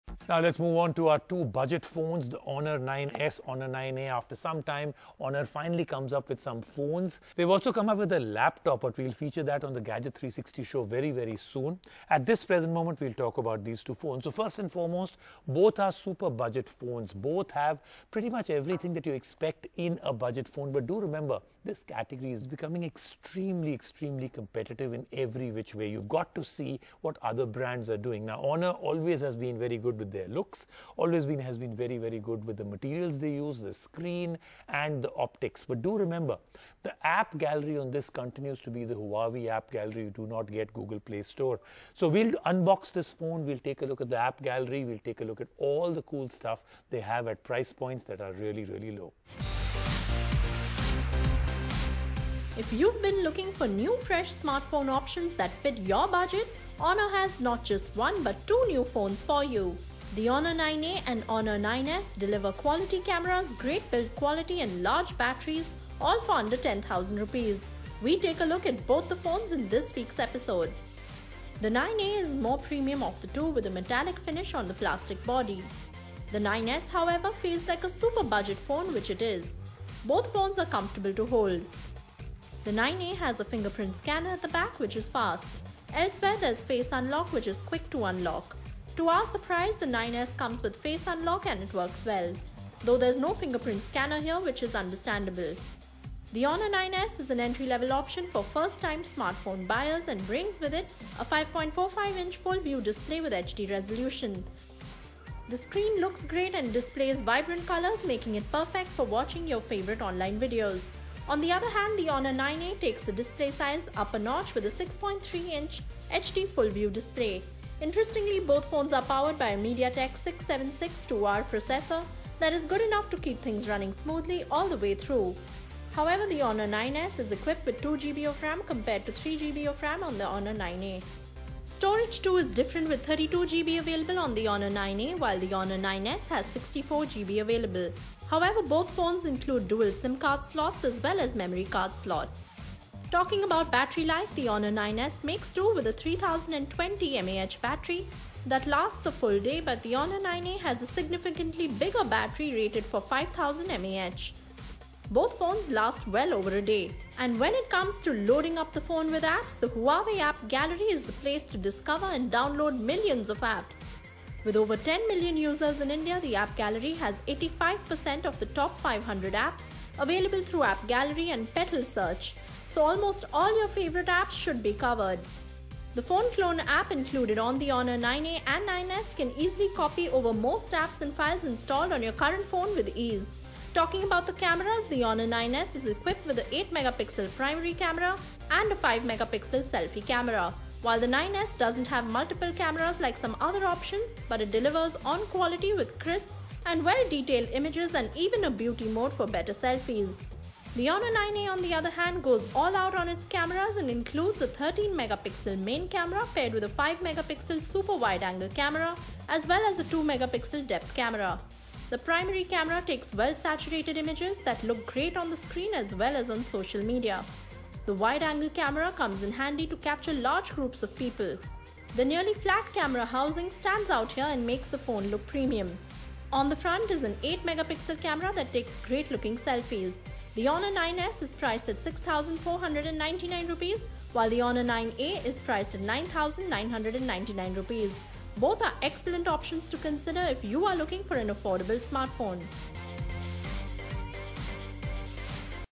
Honor's Super Budget Twins
Honor this week launched not one but two phones in the budget segment in India. Both the Honor 9A and the 9S come packed with features but have Huawei's AppGallery instead of the Google Play store. What do you get, what do you miss, and should you buy the new budget phones? We tell you.